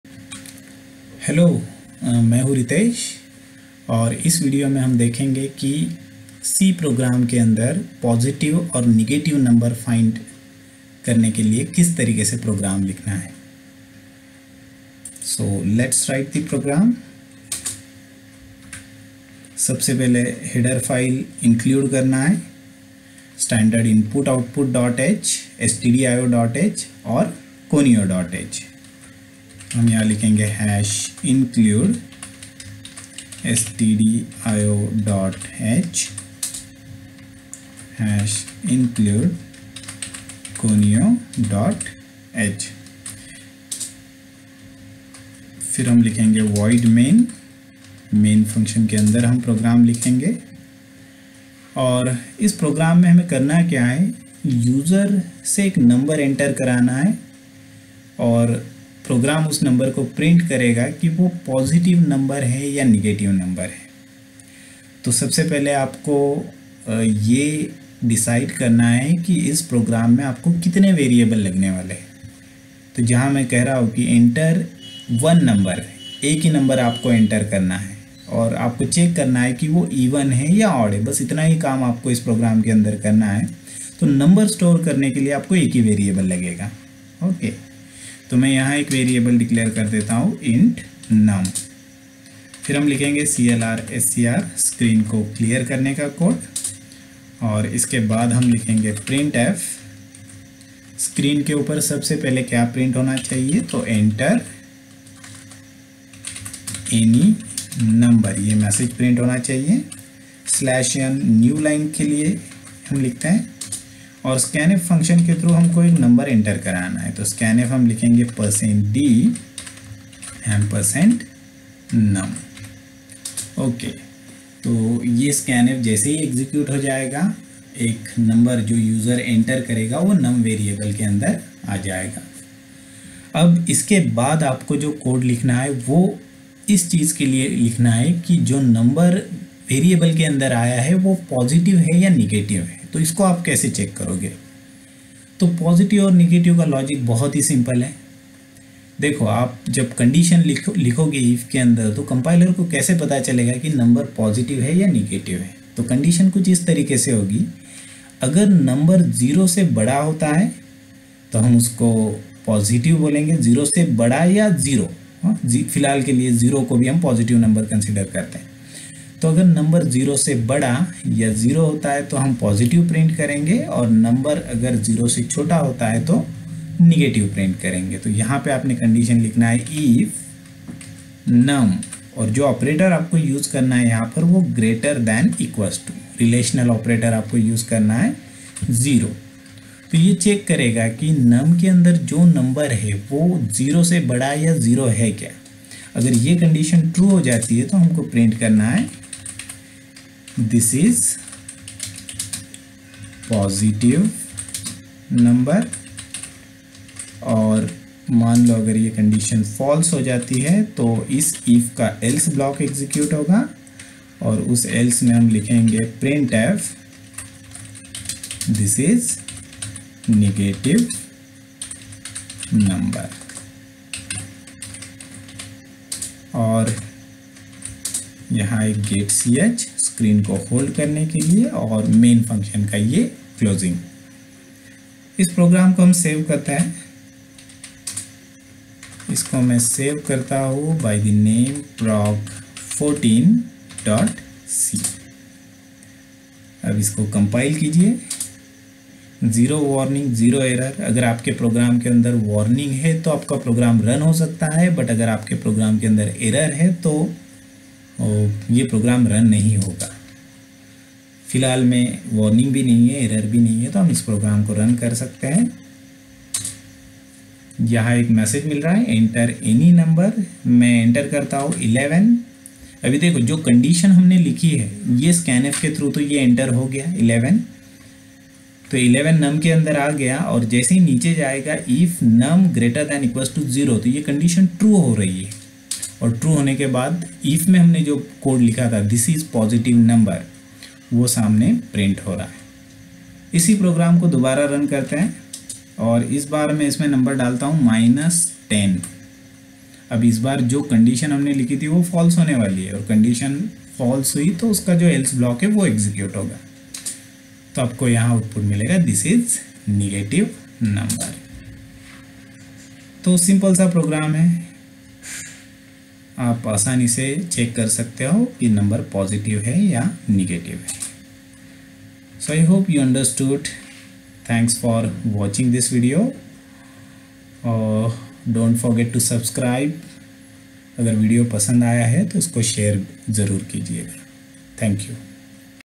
C Program to print Positive or Negative Number in Hindi 14
Ritesh,Computer science,This video will help you to understand how to  print positive or negative number in c language.

About: This is you tube channel where you will find all computer science subjects videos